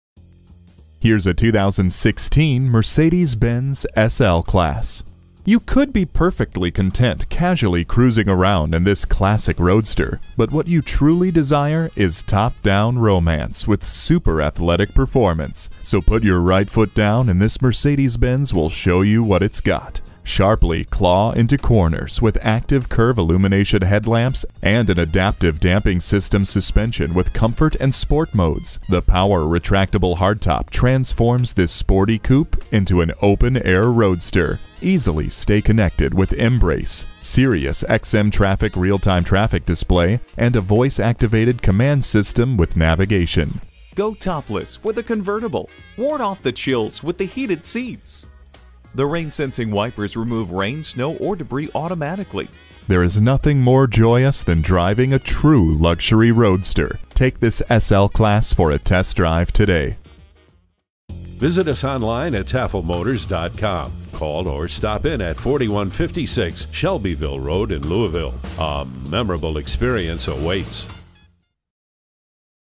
2016 Mercedes-Benz SL Louisville KY Elizabethtown, KY M15658 - SOLD
Year: 2016
Make: Mercedes-Benz
Model: SL
Trim: SL400 Roadster
Engine: 6 Cyl. 3.0 L
Transmission: Automatic
Color: Mars Red
Interior: Silk Beige/Espresso Brown
Stock #: M15658
VIN: WDDJK6FA4GF041732
The sophisticated body structure of the SL-Class is instantly recognizable yet startlingly fresh. From the upright grille with its central Star, the powerful contours of the long hood sweep past the cockpit into an aggressively contoured tail. A power retractable hardtop folds into the trunk and is triggered by a single button on the console. Occupants enjoy an expansive view of the sky through a heat-reflecting transparent panel. LED daytime running lamps and taillamps also come with the SL-Class. Each trim is powered by a different engine including a 3.0L V6, a 4.7L V8, a 5.5L V8, and a 6.0L V12 Lavished in soft, smooth exclusive leather, the cabin keeps its two occupants feeling coddled, connected, and captivated. The driver-seat features 12-way power adjustable seats with memory and lumbar support. Furthermore, the seat heating function will take the chill out of even the coldest day. Separate left and right temperature controls allow the driver and passenger to enjoy personalized comfort. All trims feature paddle shifters, allowing you to manually upshift and downshift the automatic transmission. A fuel-saving new ECO Start/Stop system automatically shuts off the engine at stoplights and other idle situations. Standard Torque Vectoring Brake technology provides additional control and precision in turns, via targeted rear-wheel braking. A suite of standard air bags offers advanced protection. Driver and passenger knee air bags supplement the dual-stage front air bags. The NECK PRO head restraints protect you in the event of a rear impact exceeding a preset threshold. Finally, Bi-Xenon headlamps help deliver greater visibility than halogen headlamps because their brighter, whiter light is much closer to natural daylight.

Address:
4156 Shelbyville Road